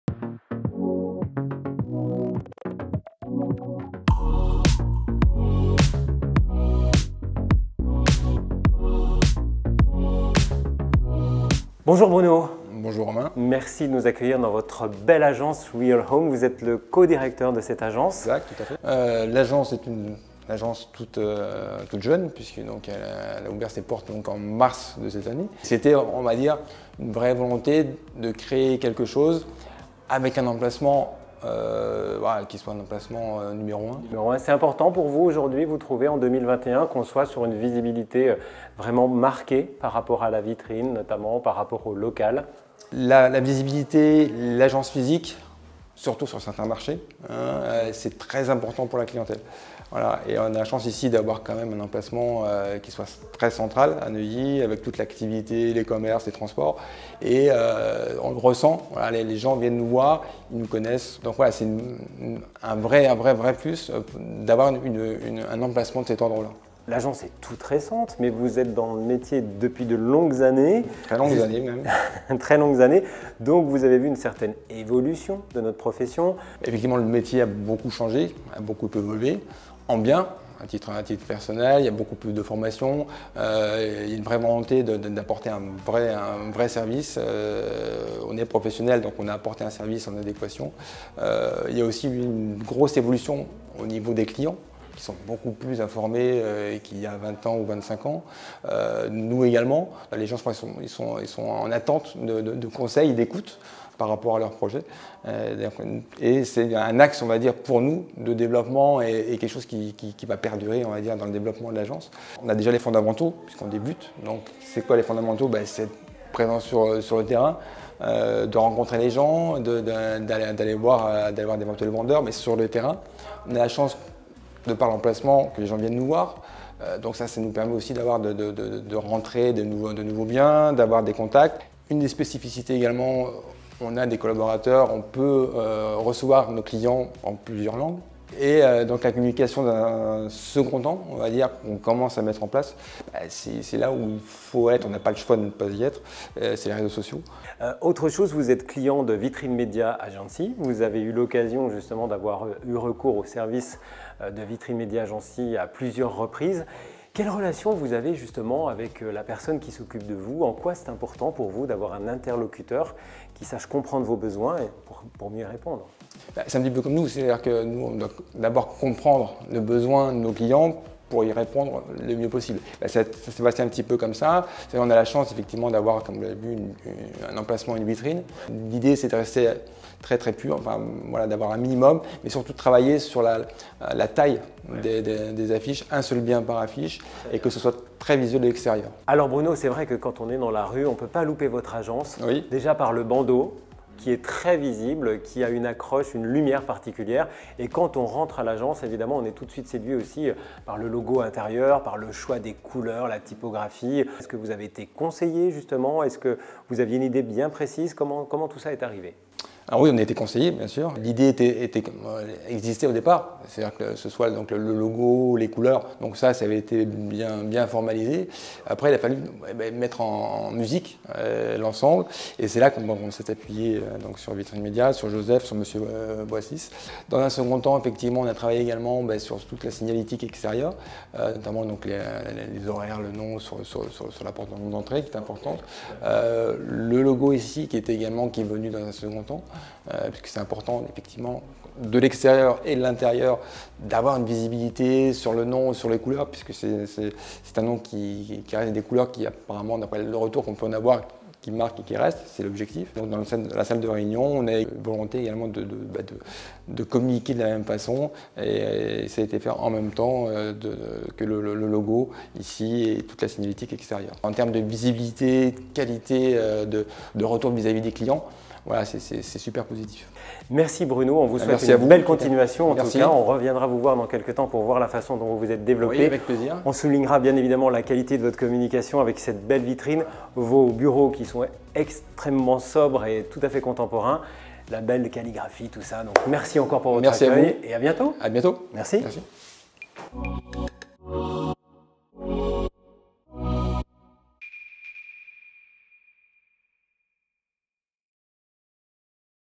🎬VITRINEMEDIA AGENCY : Real Home
VITRINEMEDIA Série : L'Expérience VITRINEMEDIA Agency de Real Home 🎬

La Mini-série des agents immobiliers continue avec l'épisode 9 : Rencontre avec Bruno Lascorz, directeur d'agence.